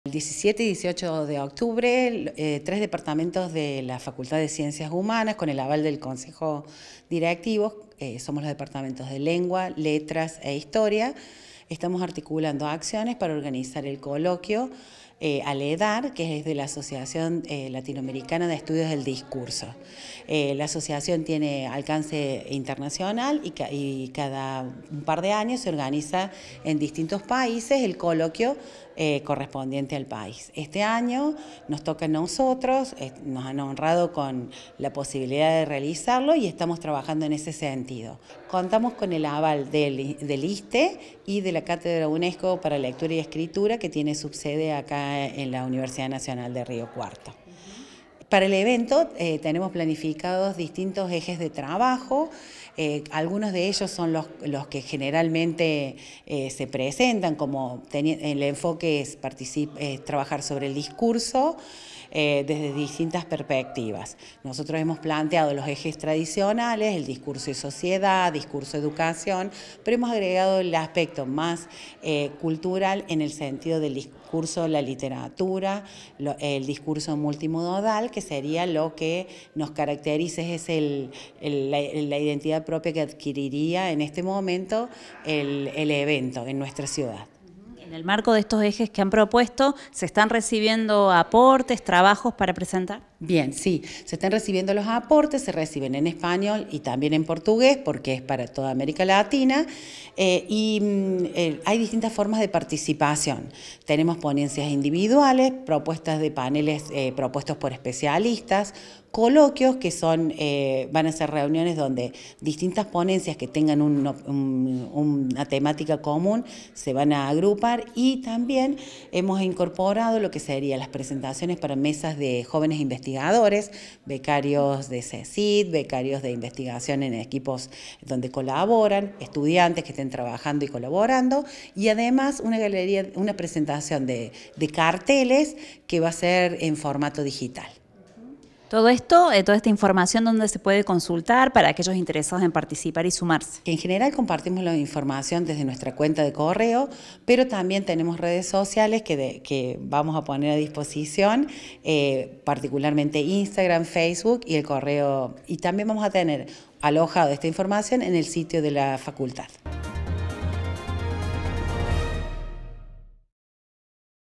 EN OCTUBRE, LA UNRC SERÁ SEDE DEL COLOQUIO DE LA ASOCIACIÓN LATINOAMERICANA DE ESTUDIOS DEL DISCURSO
RENATA CARDINALI – DOCENTE DPTO LENGUAS
UniRio TV
Universidad Nacional de Río Cuarto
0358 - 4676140